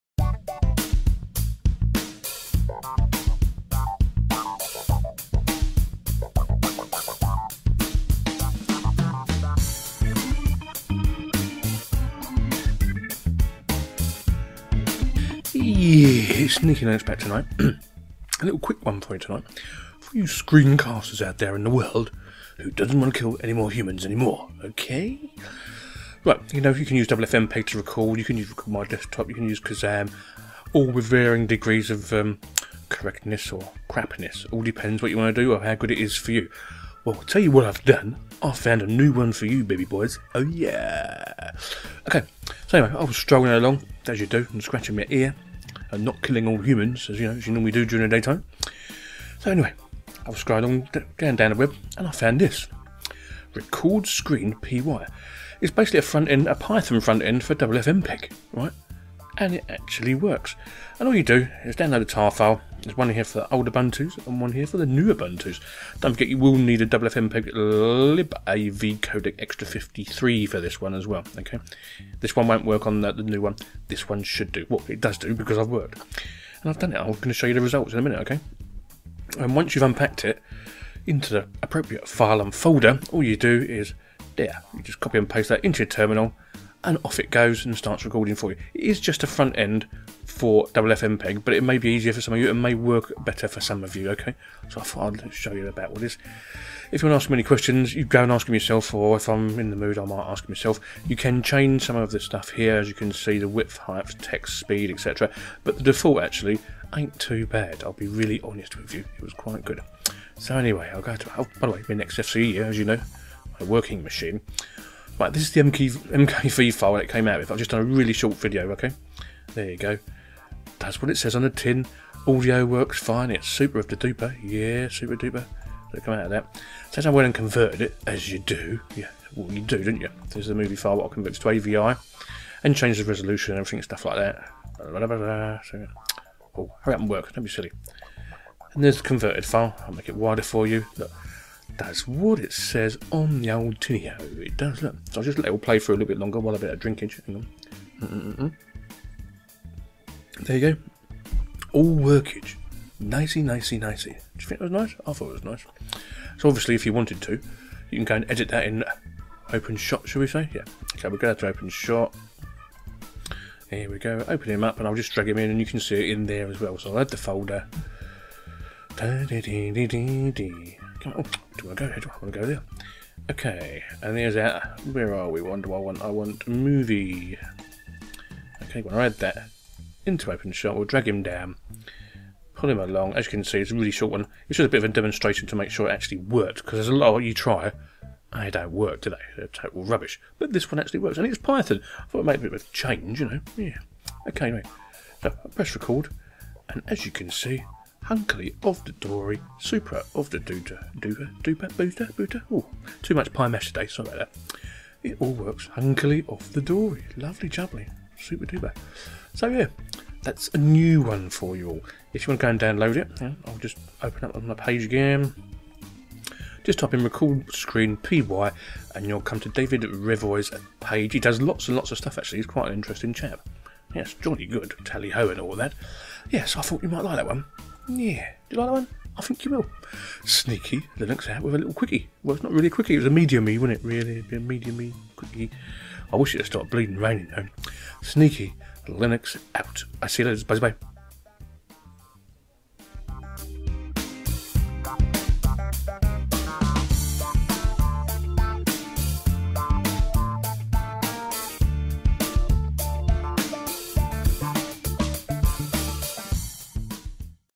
Recordscreen.py.....Ffmpeg With Python.
Here is one for all you screencasters who may be having probs or just wanna try somthing different and it's called screenrecord, and its been around for a little while so go give it a try. bumper!!